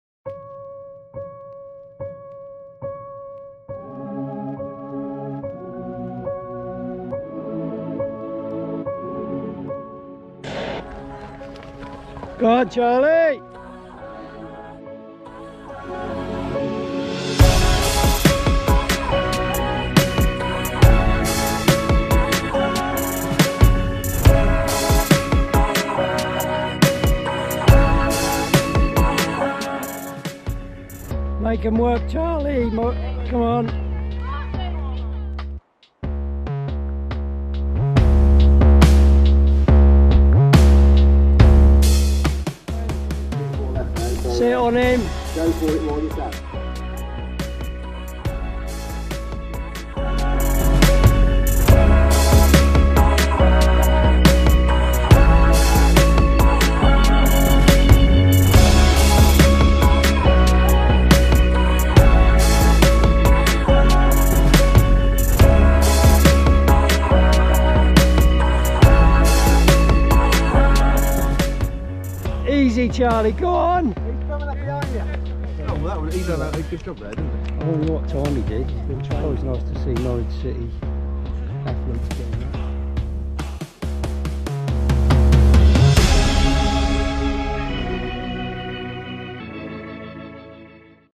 Norfolk County Championships | 5000m | 2024
Winner, Charlie Wakefield in 14.59